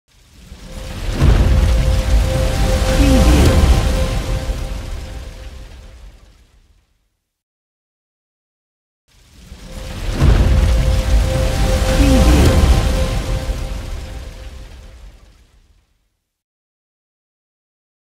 Logo Fire Reveal
Logo Fire Reveal is an eye-catching After Effects template with a hot and fiery looking logo reveal. It's so easy to use with 1 logo placeholder, 1 editable text layer and an intelligent color controller. A wicked intro or opener to your TV shows, commercials, presentations, slideshows, promotions and events videos. Impress your audience with this bright and dynamically animated AE template. No plugins required.